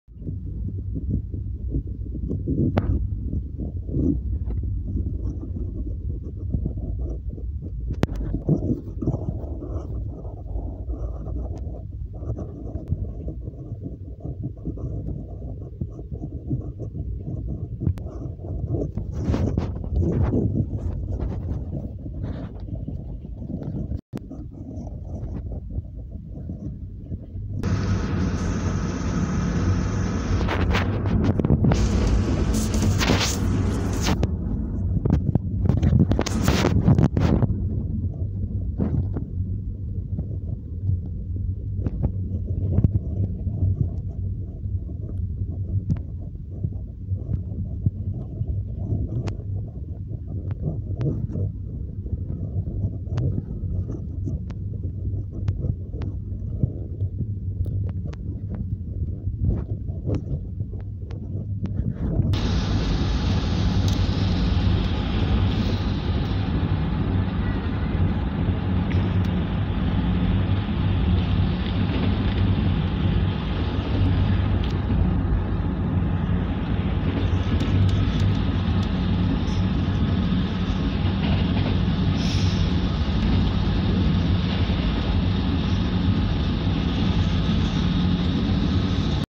Eye-catching Sunset .. Train dairies.. sun and I on the same journey..ready to rise again.
travelling by train and observing the beautiful sunset near Gujranwala city of Pakistan